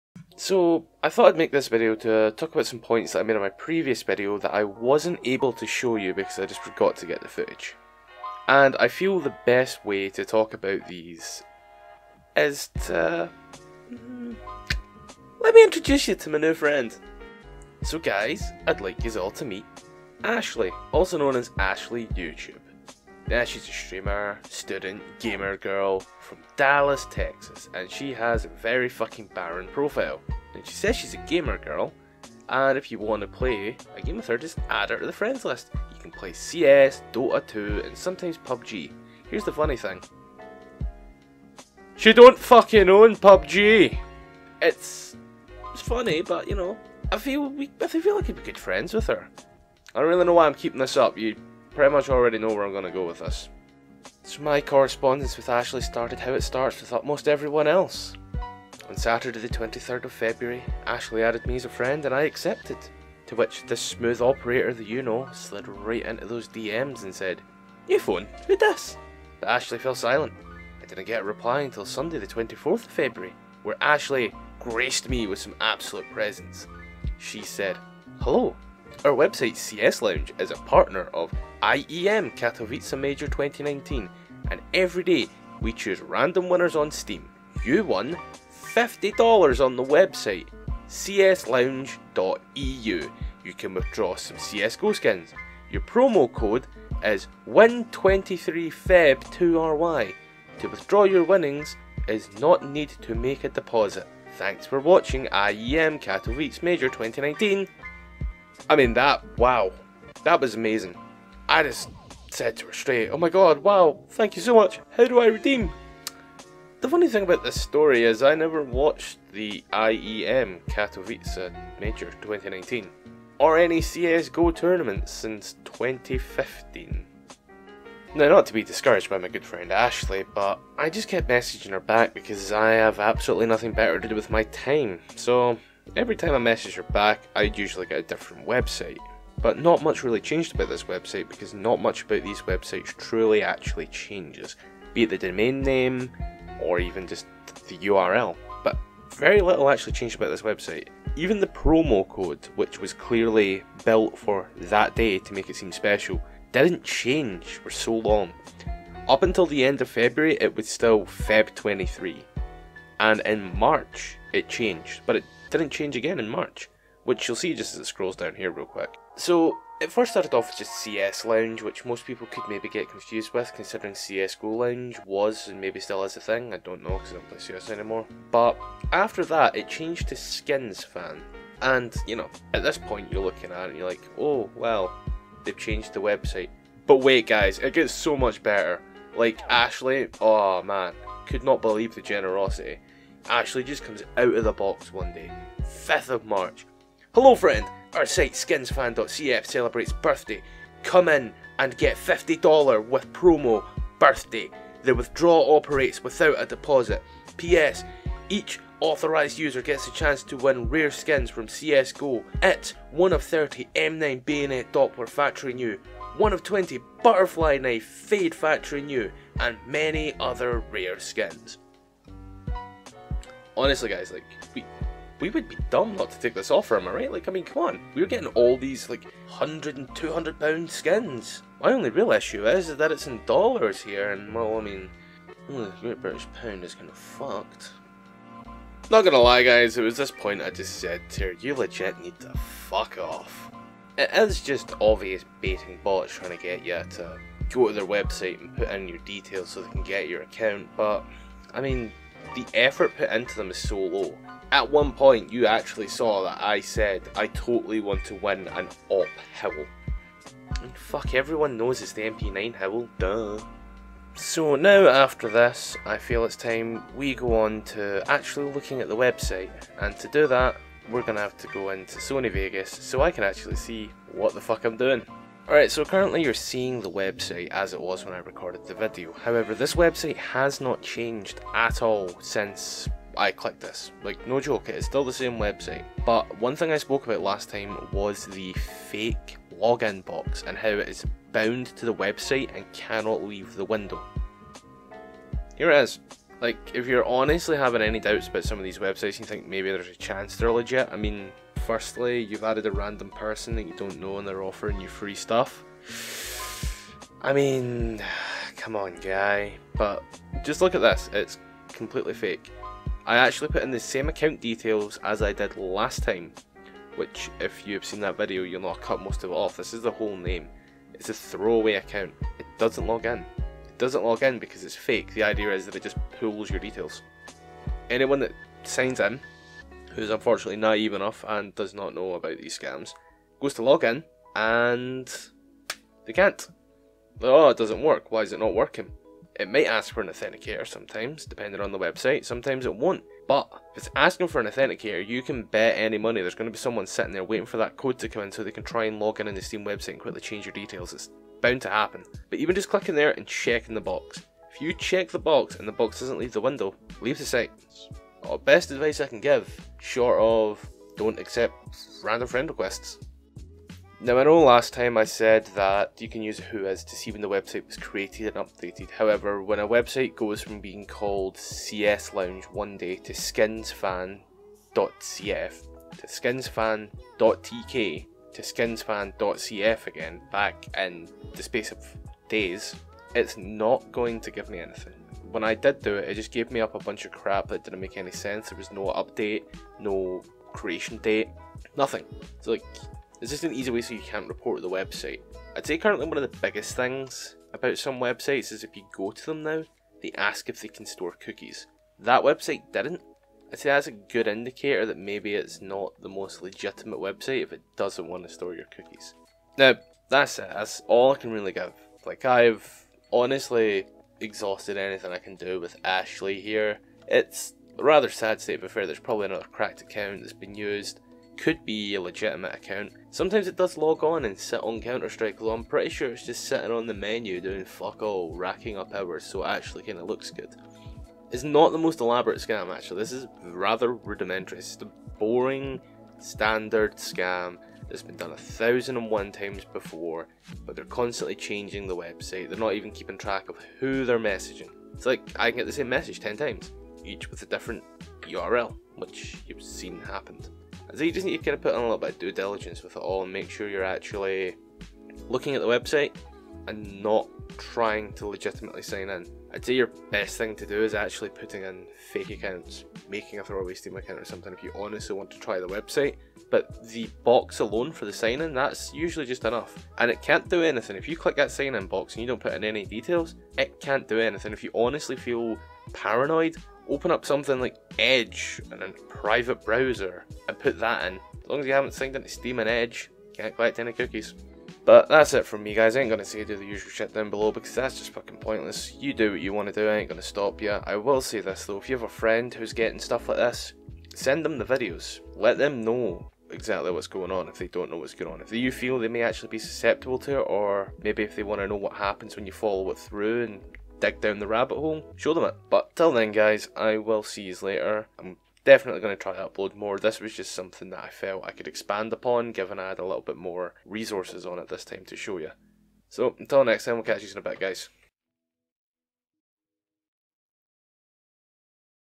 [UPDATE] Steam inventory Scam 2019 followup
There were a few things I didn't get to put in the last video that this time I was able to actually capture as well as a funny little story of the worst scam I've seen to date.
Hope yous enjoy and learn something.

All Social & Credits below